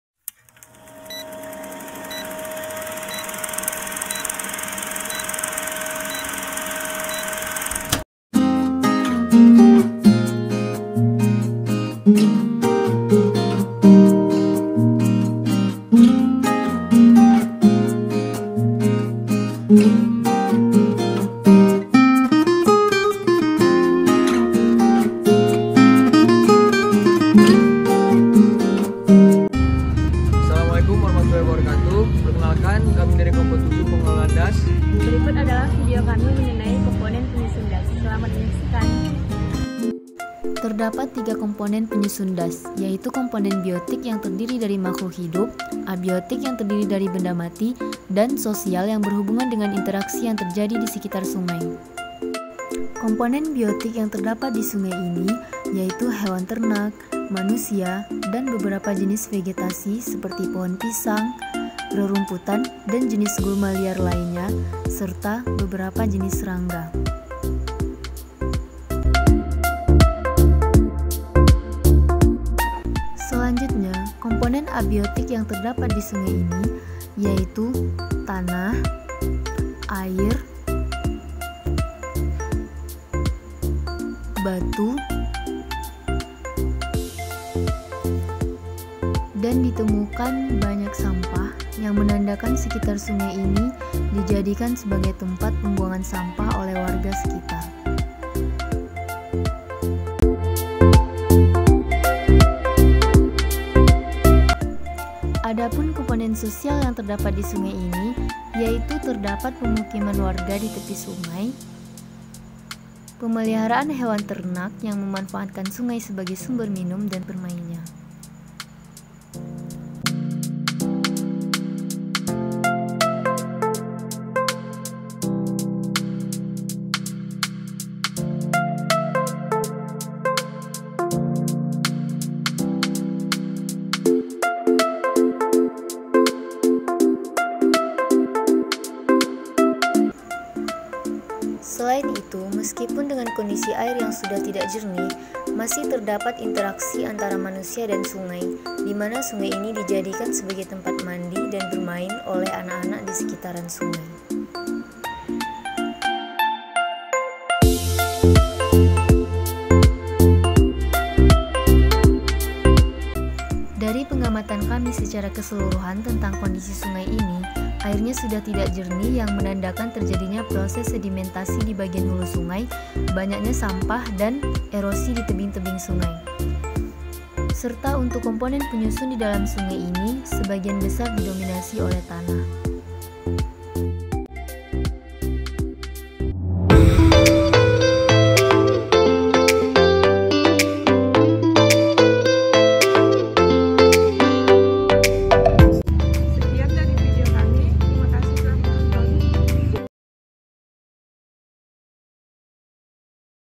KOMPONEN PENYUSUN DAS DI SUNGAI DELI, KEC. NAMORAMBE_KELOMPOK 7_ILTAN 1_FP-USU
Mata Kuliah : Praktikum Pengelolaan DAS
Dosen Pengampu : Jamilah, SP. MP

KOMPONEN PENYUSUN DAS
Tujuan Praktikum : Untuk mengetahui komponen penyusun DAS di Sungai Deli, kec. Namorambe, kab. Deli Serdang.

Anggota Kelompok 7 :
1. Tasya Maidana Chikita (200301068)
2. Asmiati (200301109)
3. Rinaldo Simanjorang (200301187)
4. Tondiaji Santoyibi Nst (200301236)

Kelas : Ilmu Tanah 1